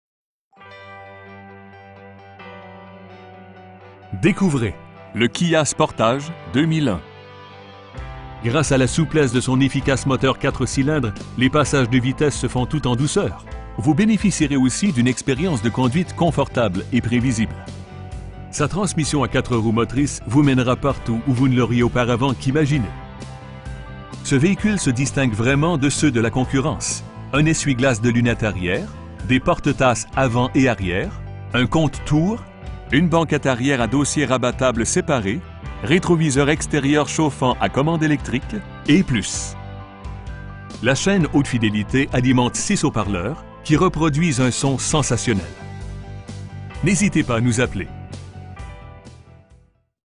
2001 Kia Sportage EX 4WD SOLD AS IS in Montr��al, QC H4B 1V2
Hyundai Prestige
6435, Saint-Jacques O.
in Montr��al, QC H4B 1V2

D��couvrez le Kia Sportage 2001. Gr��ce �� la souplesse de son efficace moteur 4 cylindres, les passages de vitesses se font tout en douceur, vous b��n��ficierez aussi d'une exp��rience de conduite confortable et pr��visible. Sa transmission �� quatre roues motrices vous m��nera partout o�� vous ne l'auriez auparavant qu'imagin��. Ce v��hicule se distingue vraiment de ceux de la concurrence: un essuie-glace de lunette arri��re, des porte-tasses avants et arri��res, un compte-tours, des si��ges baquets �� l'avant, une banquette arri��re �� dossiers rabattables s��par��s, r��troviseurs ext��rieurs chauffants �� commande ��lectrique, et plus. La cha��ne haute fid��lit�� alimente 6 haut-parleurs, qui reproduisent un son sensationnel. Notre fiert�� se manifeste �� travers l'excellent service �� la client��le que nous offrons. N'h��sitez pas �� nous appeler.